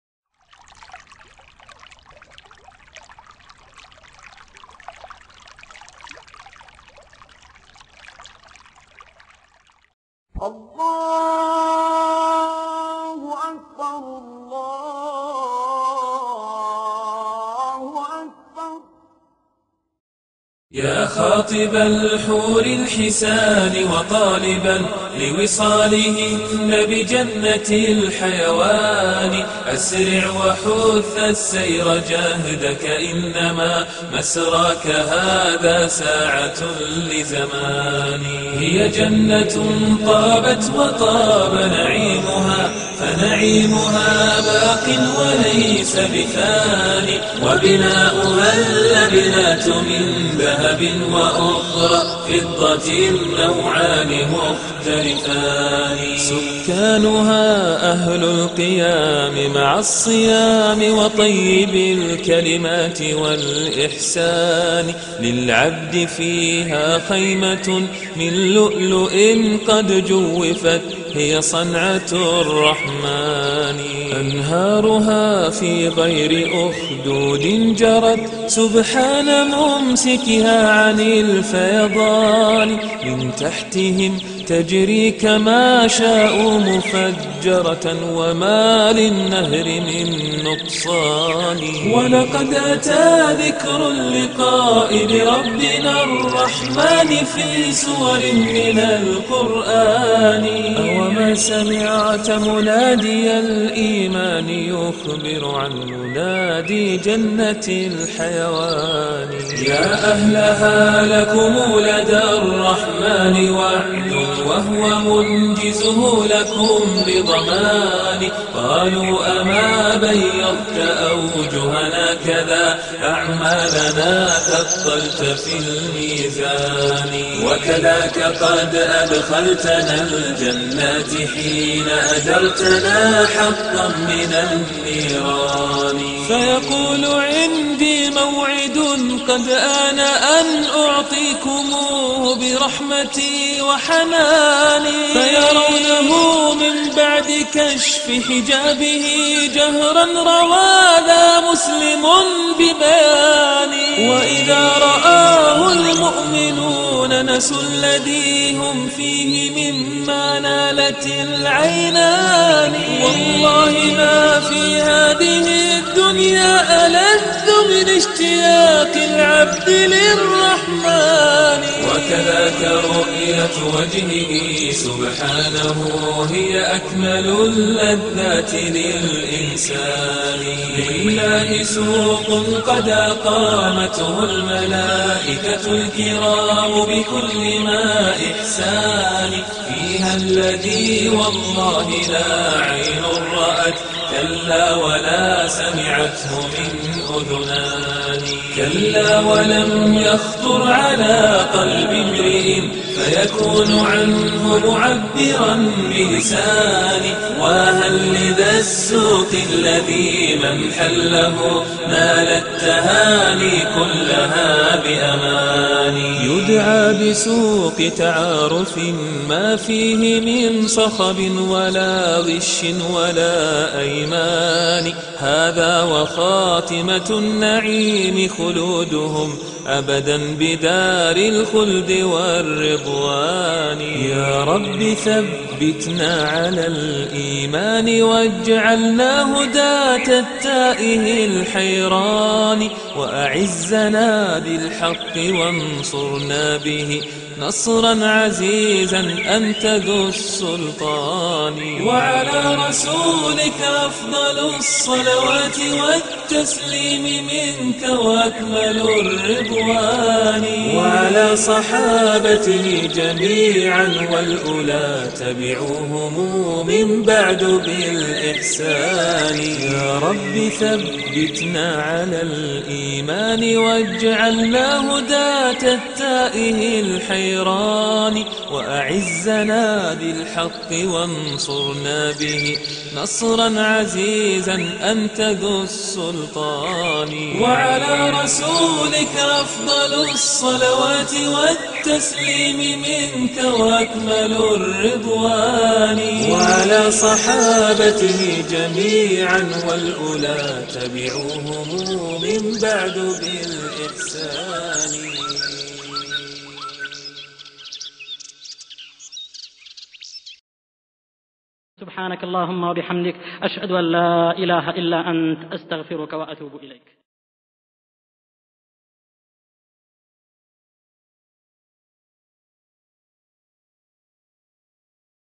Poem: The Description of Paradise by Ibn Al-Qayyim (Al-Sirat Al-Mustaqeem)
The following  verses are part of the Nuniyah of Ibn Al-Qayyim (May Allah shower him with Mercy), also known as (الكافية الشافية في الانتصار للفرقة الناجية) 'Al-Kaafiyah Al-Shafiya fi al-Intisaar lil Firqa Al-Najiya'. The poem is over 6,000 verses.

In these particular verses, Ibn Al-Qayyim (May Allah shower him with Mercy) gives a beautiful description of some of the rewards and pleasures which Allah has prepared for the Believers in the Hereafter, as was mentioned in the Quran or in Prophetic Traditions.

Our Contribution to this video has been editing a translation of these verses available online and then adding it to the video. May Allah reward everyone who contributed to this video with the best of this life and the hereafter, ameen.